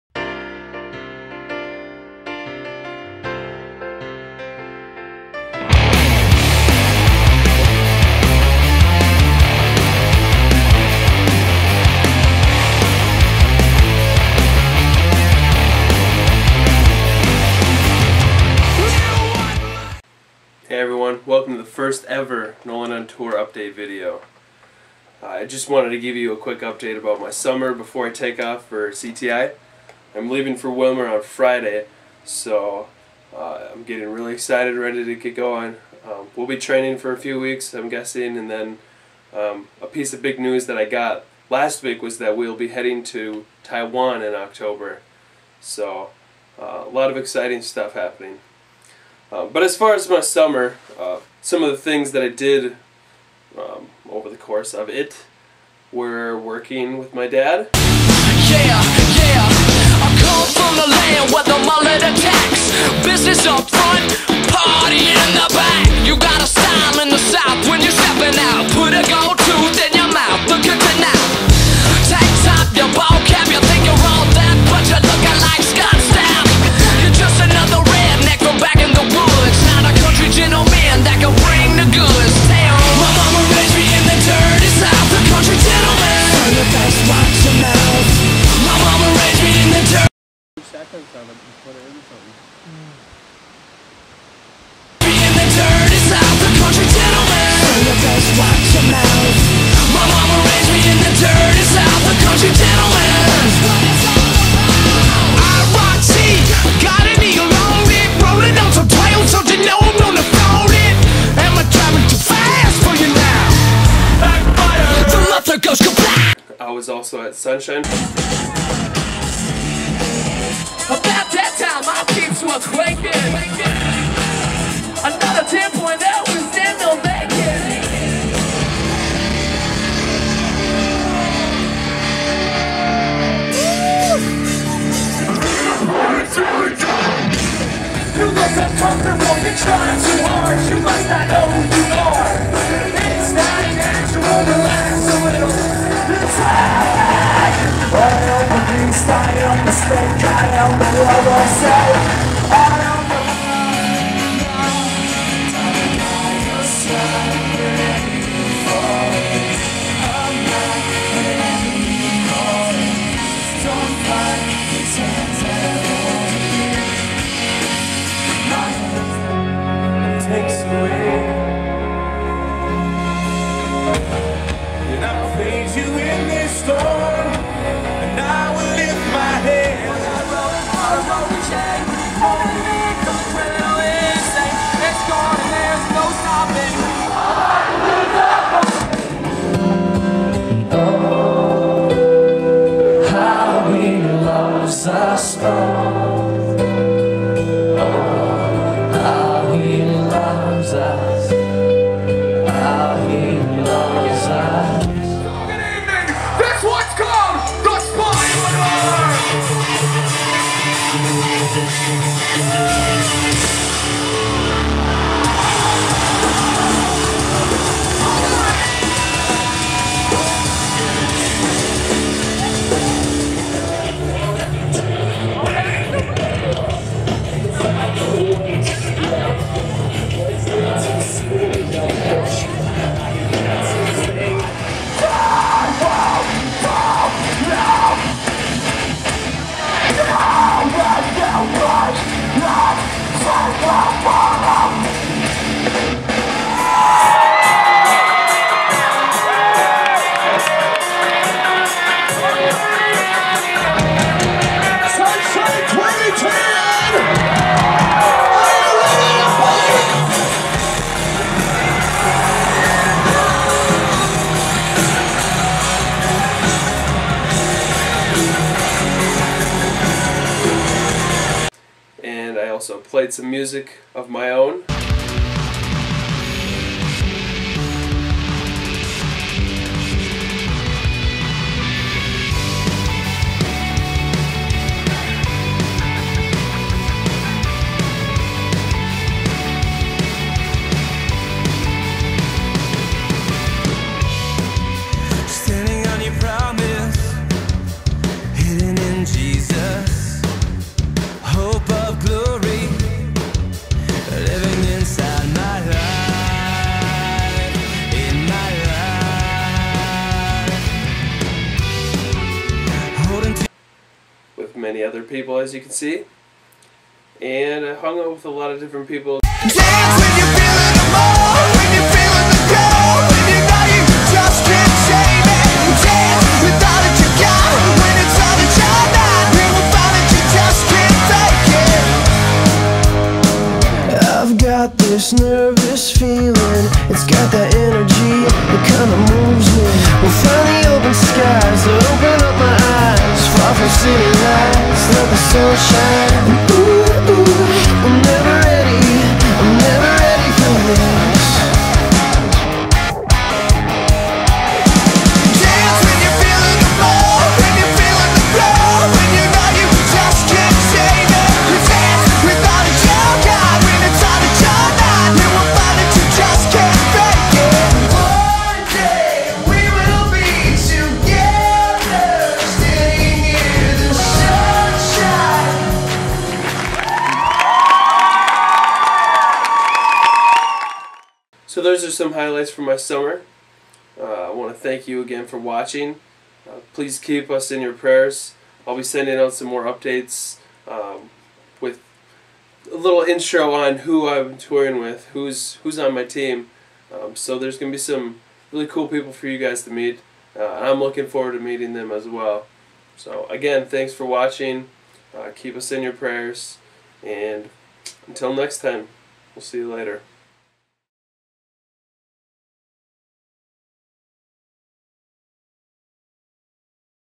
Summer Update - Nolan On Tour - CTI
Wanna know what I did this summer?  This vid will give ya the scoop.  and see what CTI has going